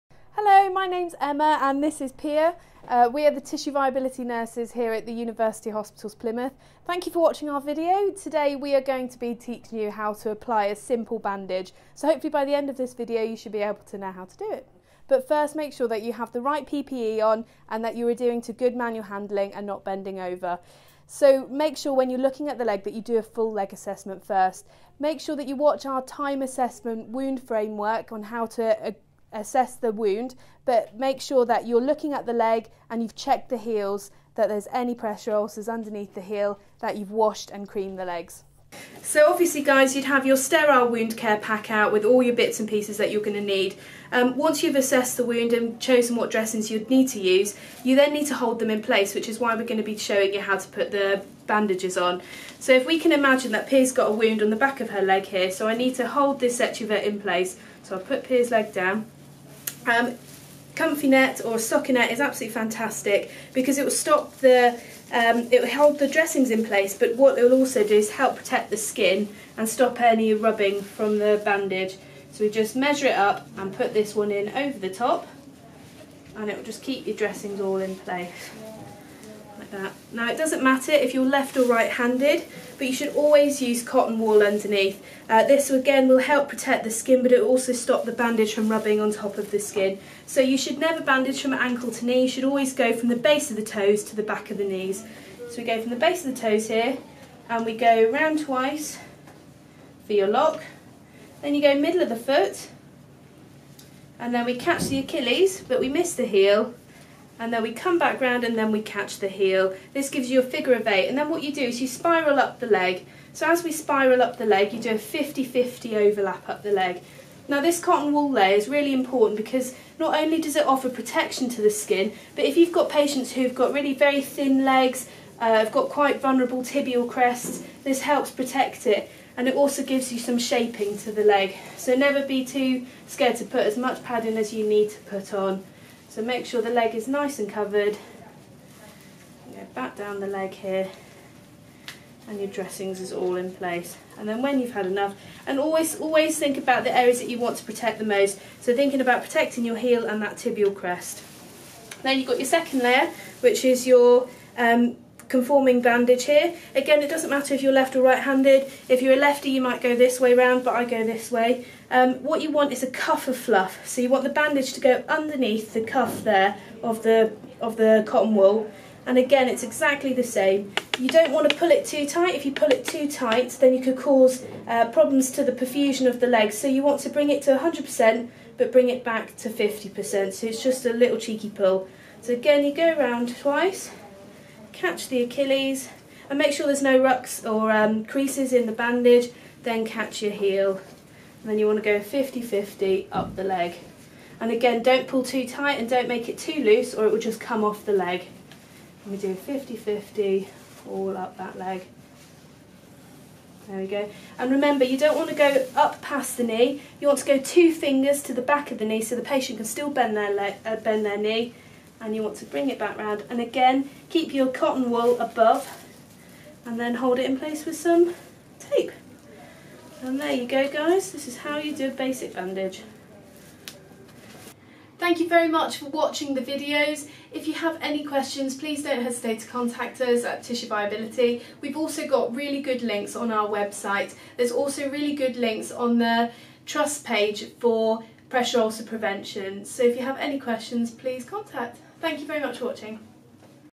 How to apply a simple bandage
Tissue Viability Nurses, Emma Walters and Pia Prince, demonstrate how to apply a simple bandage for your patient.